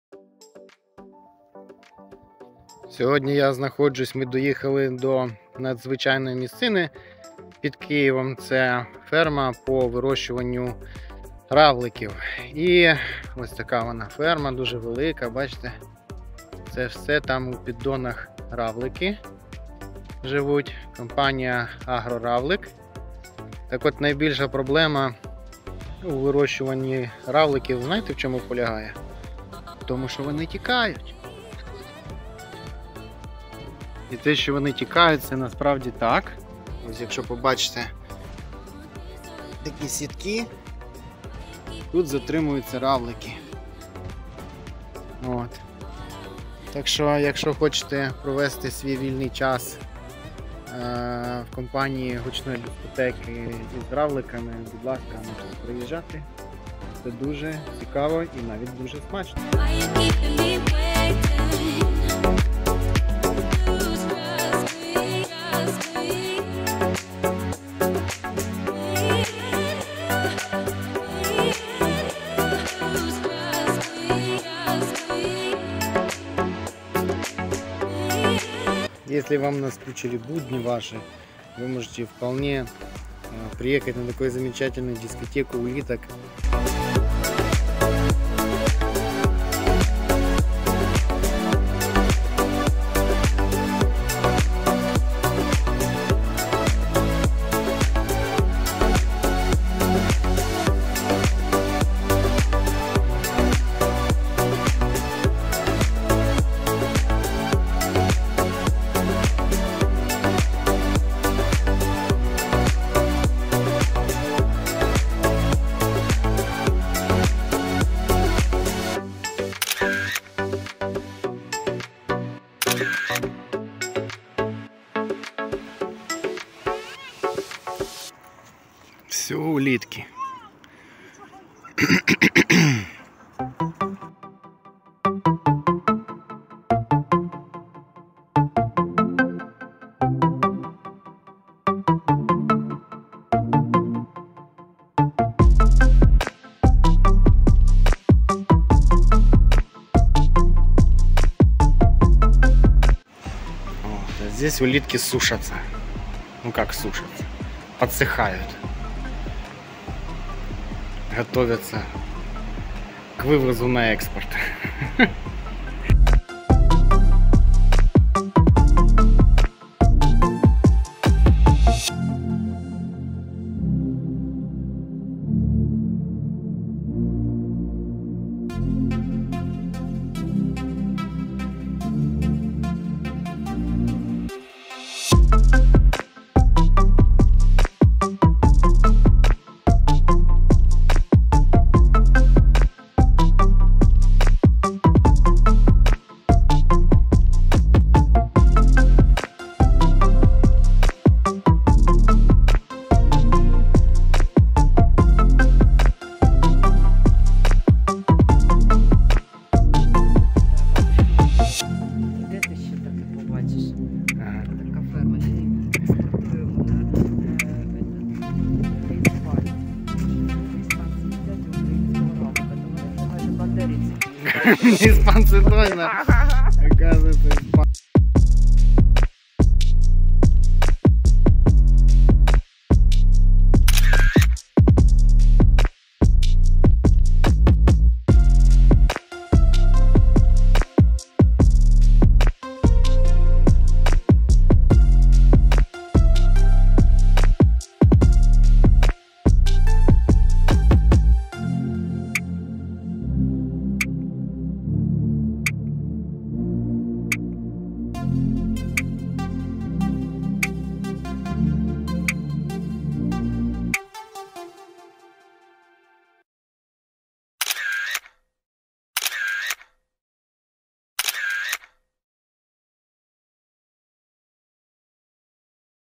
Ферма по выращиванию улиток, полный выпуск. Агроравлик
Недавно я посетил ферму по выращиванию улиток, что под Киевом. Ферма производит продукцию на экспорт в страны Евросоюза и на внутренний рынок. На ферме можно попробовать блюда из улиток и интересно провести время.